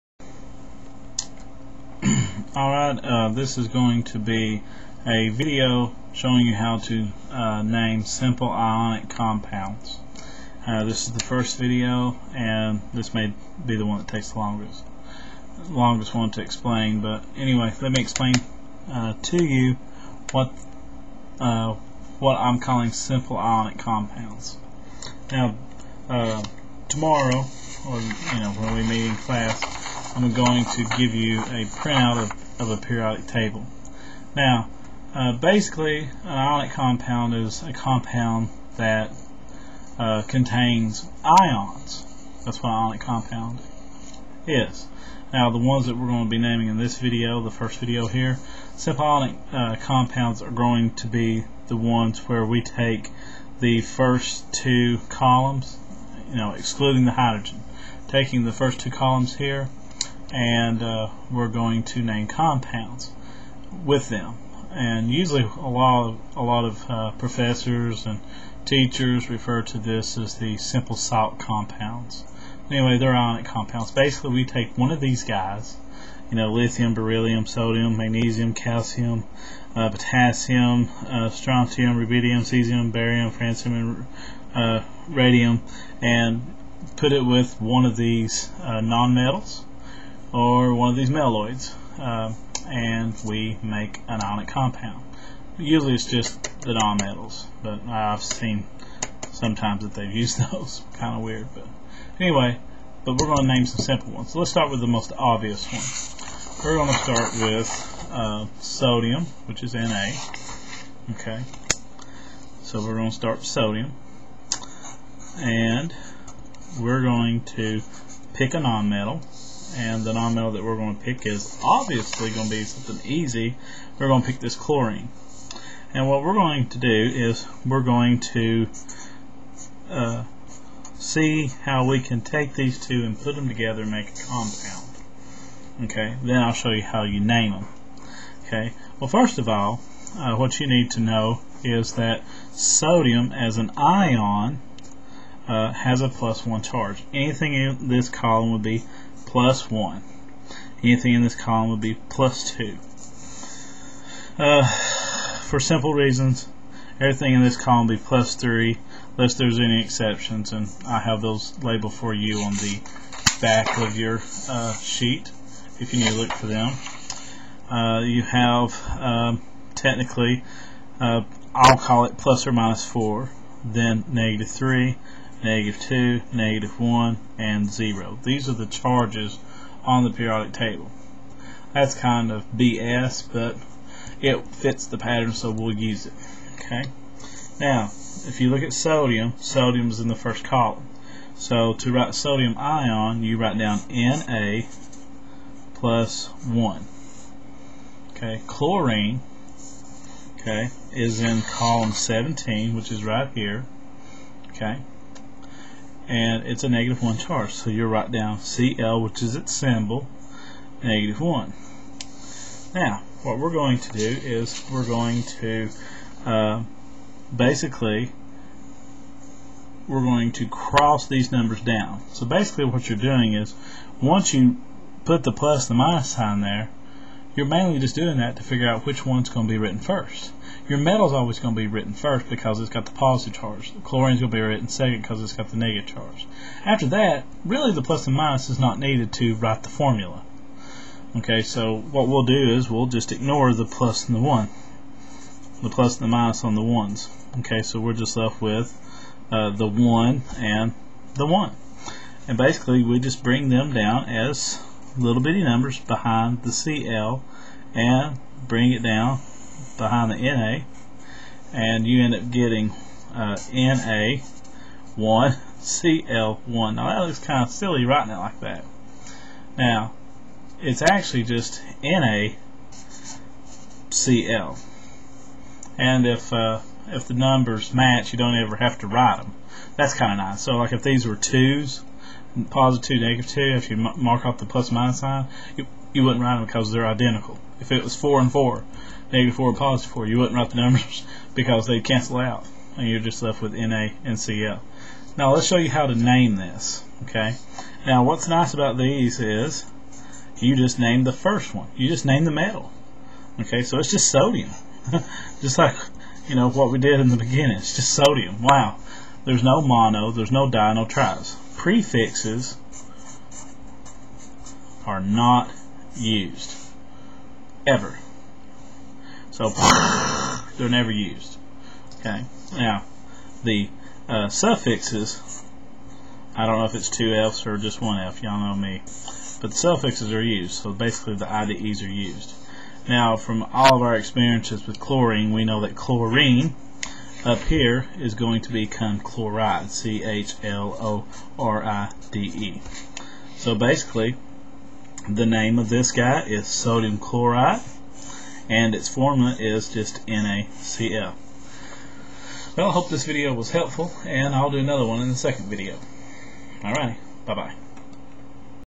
Naming Simple Ionic Compounds Video #01
Naming Simple Ionic Compounds Video #01 | Chemistry Whitwell High School UTC - the University of Tennessee at Chattanooga Instructor/Professor: Johnny Cantrell